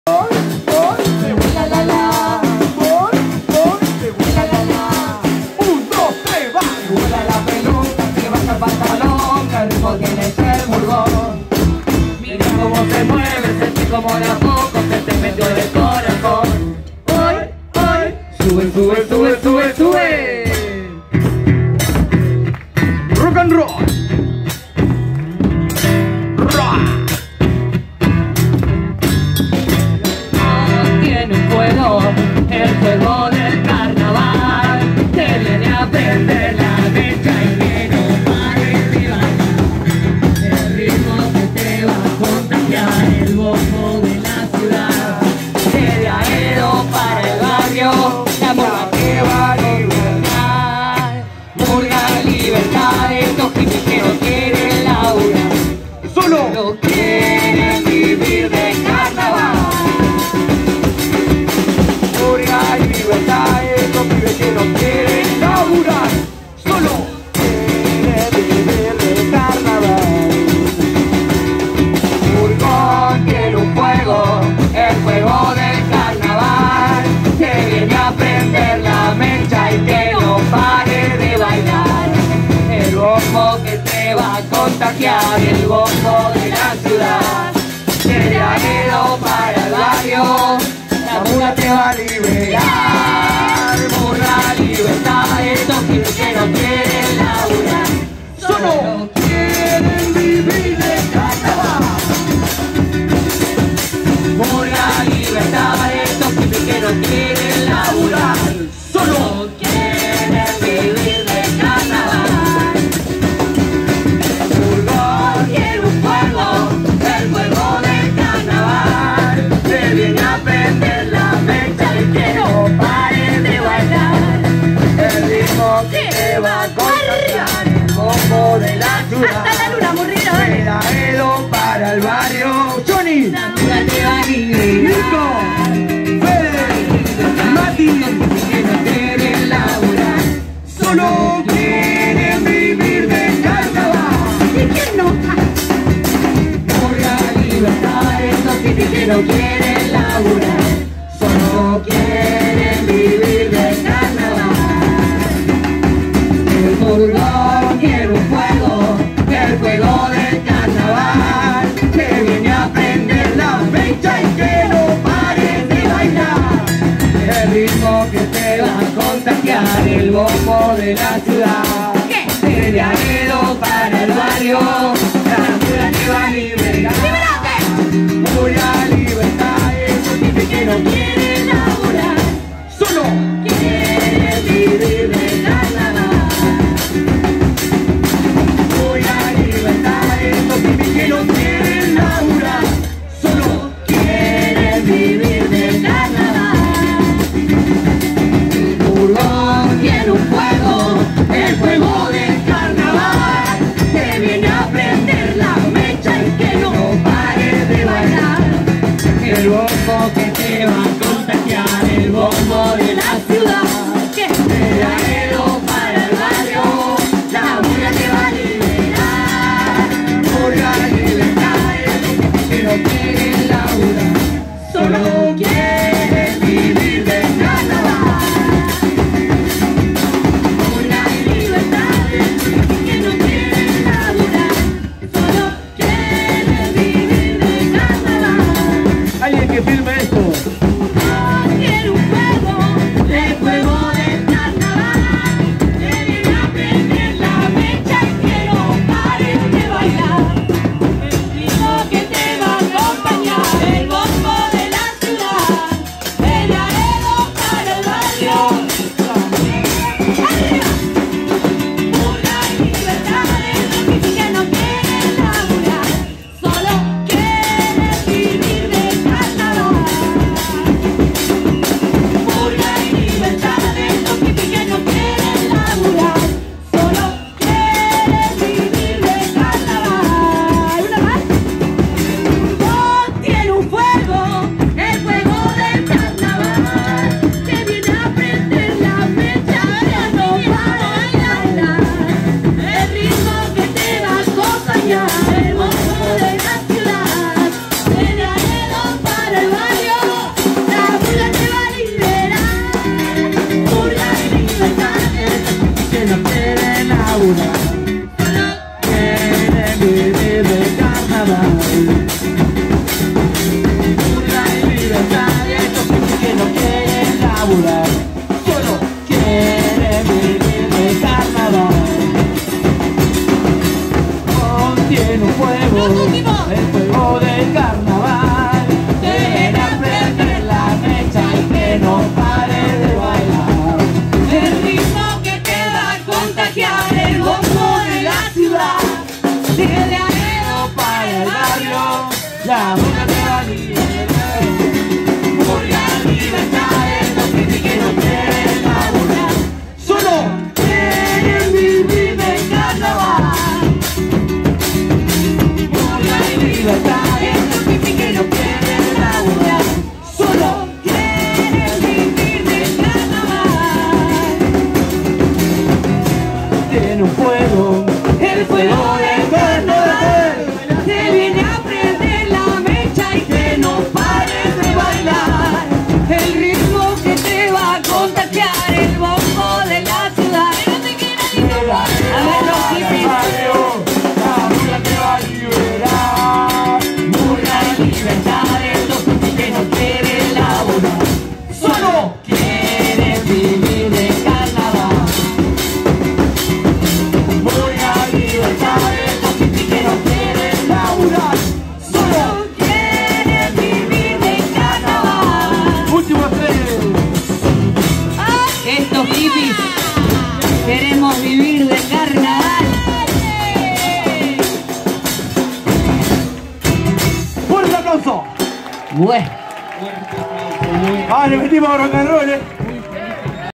Matanza 🟡🔴 Cosa e' Mandinga ⚫🟢 - Corso Cosa e' Mandinga - CARANAVAL 2022 🥳🥳
Demostración de baile de Cosa e Mandinga en su propio corso. 🥳🥳   Toda una fiesta 🥳🥳